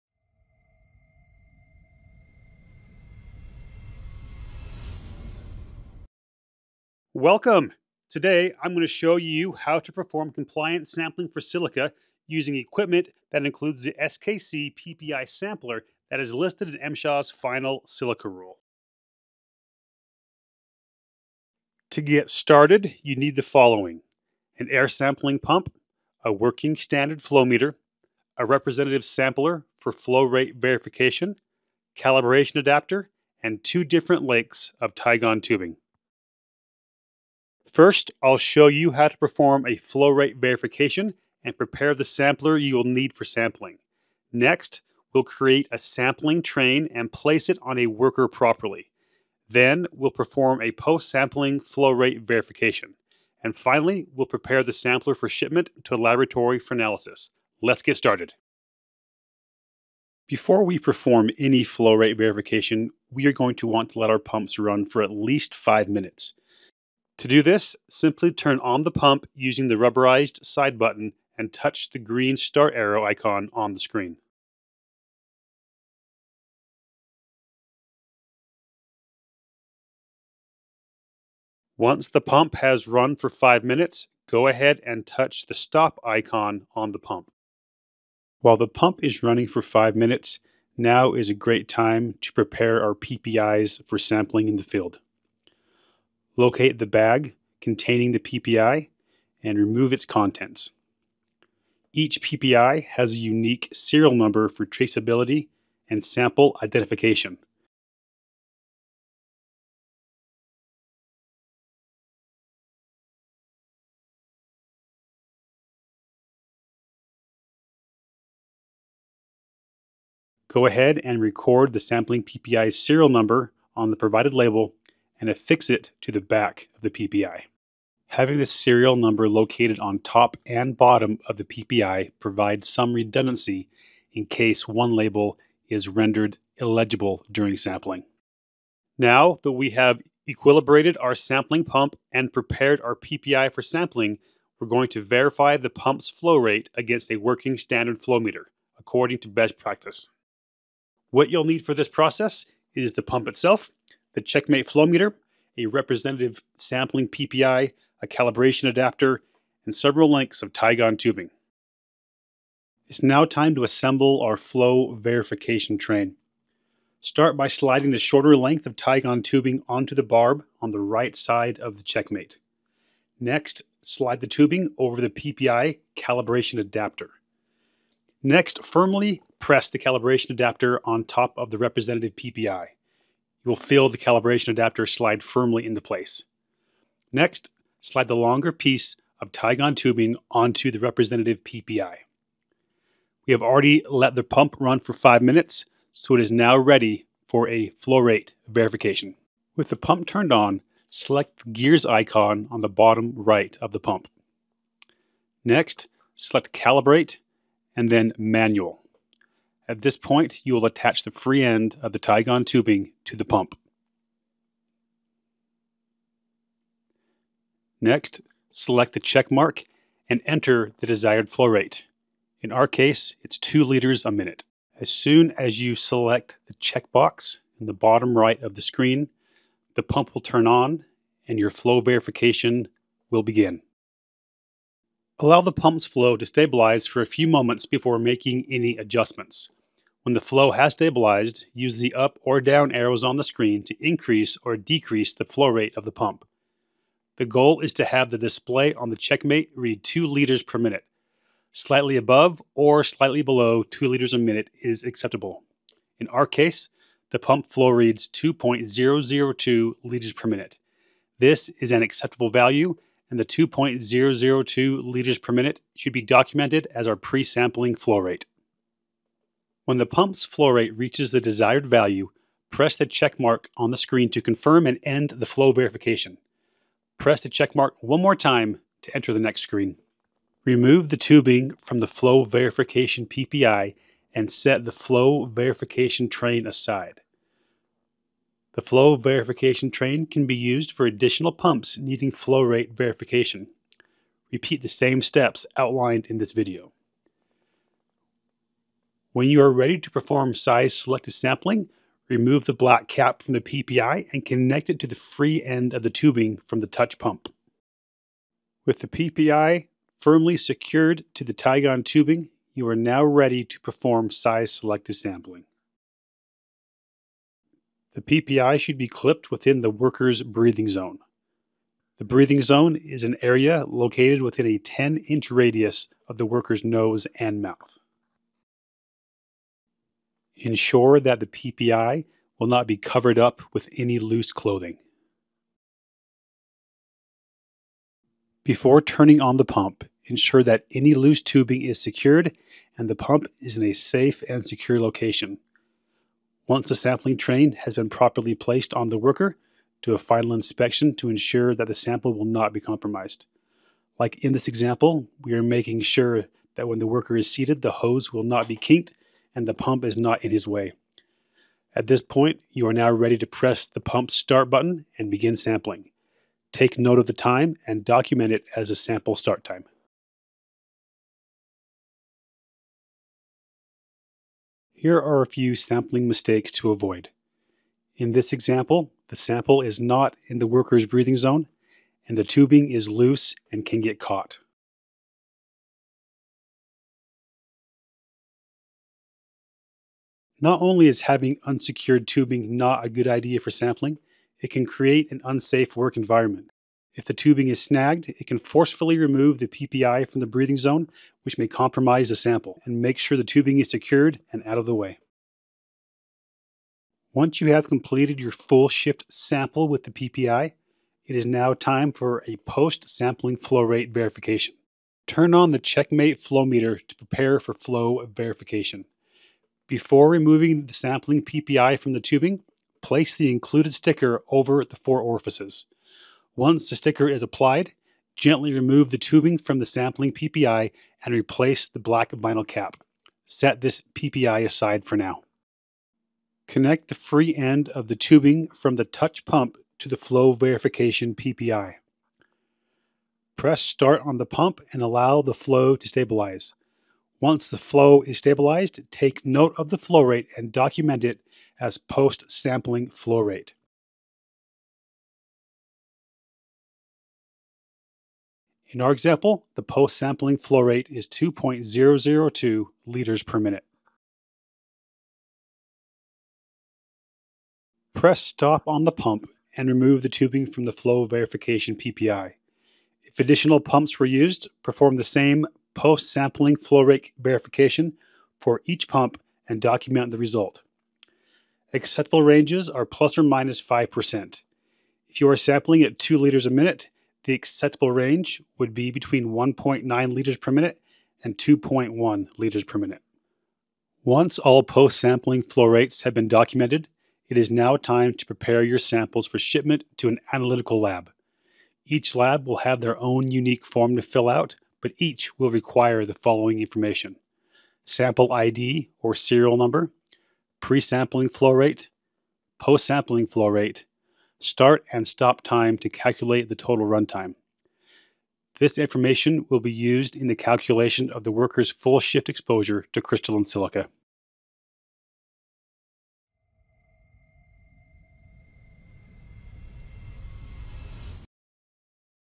How to Sample for Silica | SKC
Want to simplify silica air sampling? SKC walks you through the process for accurate and compliant sampling to meet OSHA and MSHA regulations. Discover SKC’s AirChek TOUCH pump, PPI Samplers, and Silica Sampling Rental Kits—all designed for user-friendly operation and reliable data.